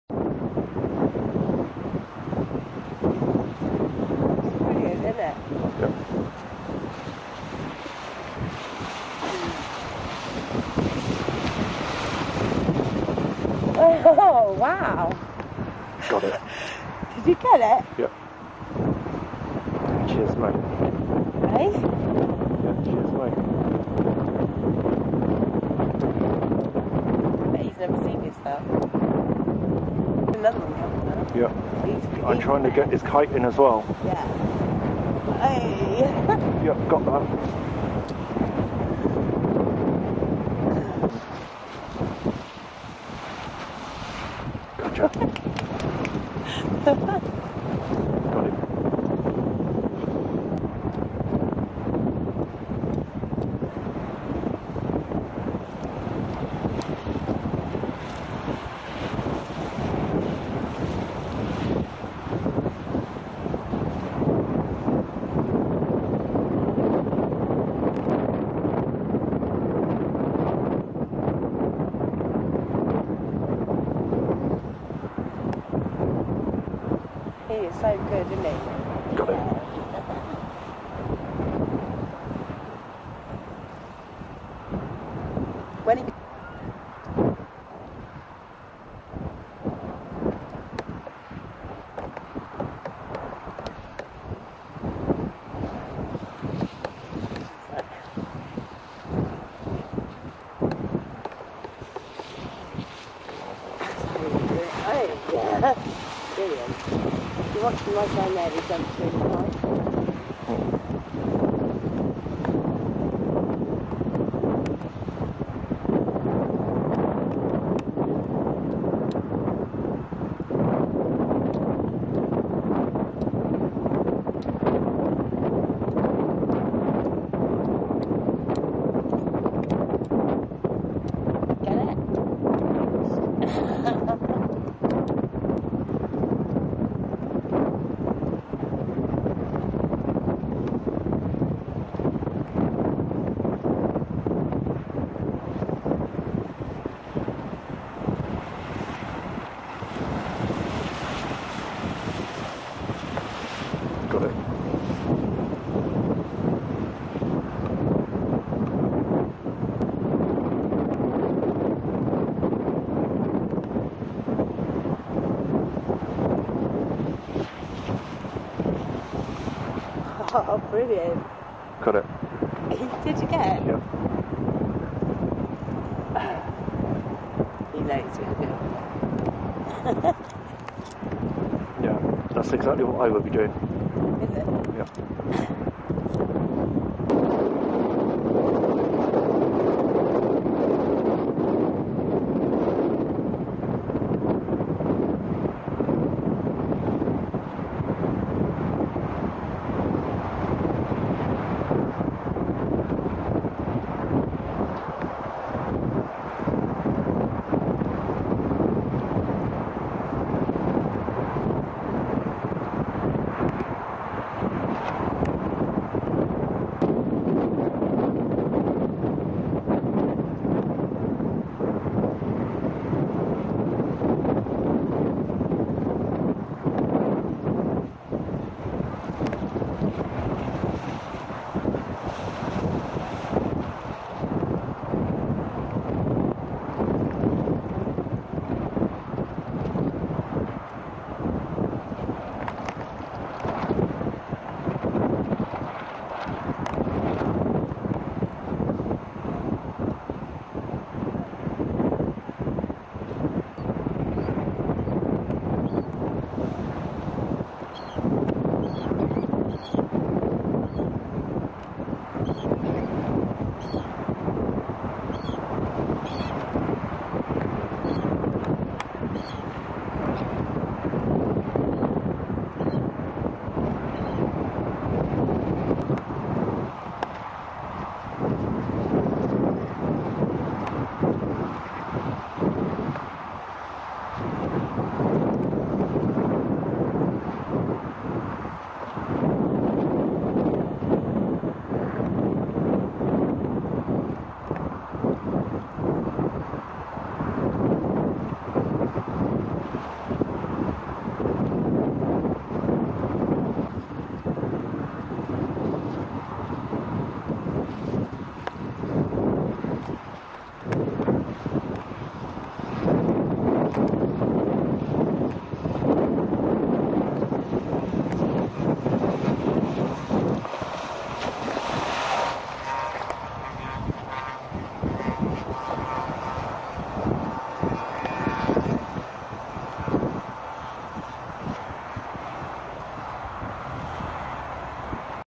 kitesurfeing.flv
Venue for Olympics 2012, Weymouth & Portland, Kite Surfing